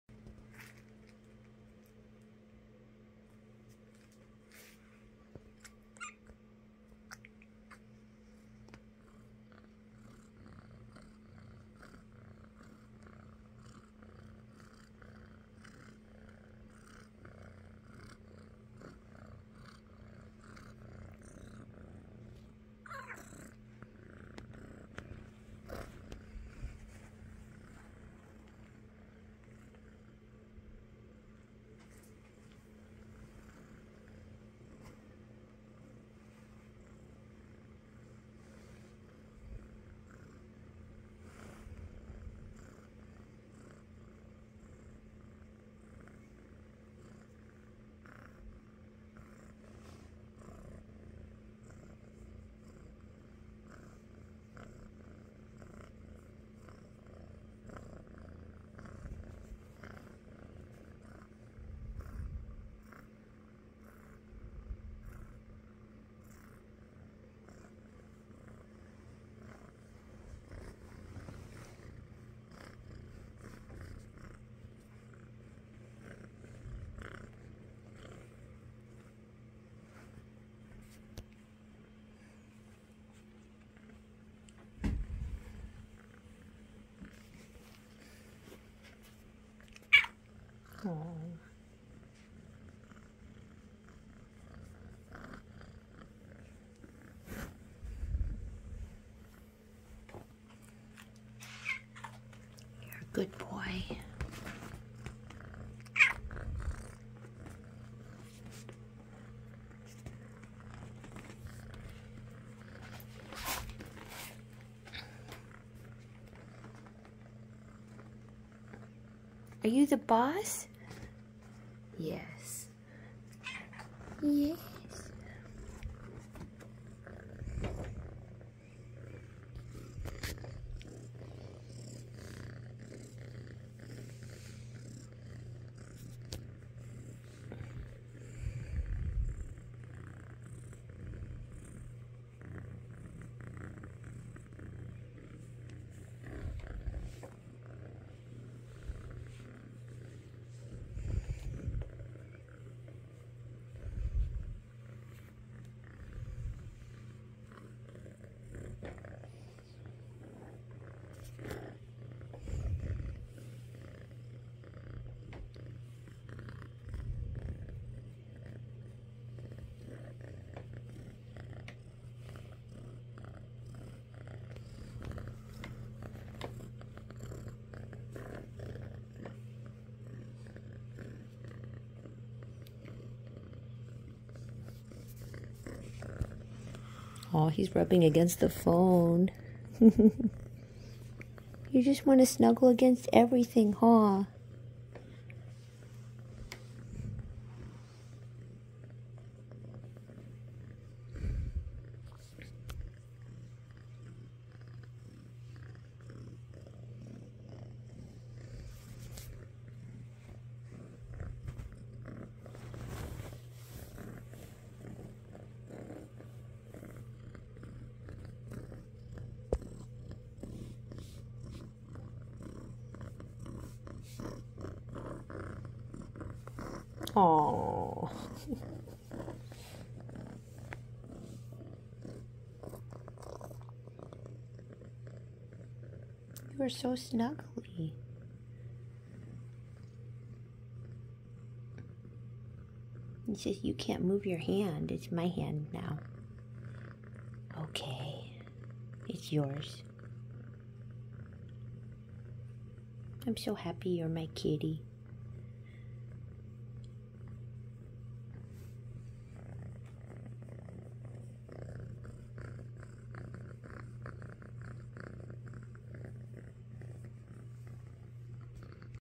What an awesome tabby
This kitten has lived with me for two months now and he brings me so much joy!  And cuddles.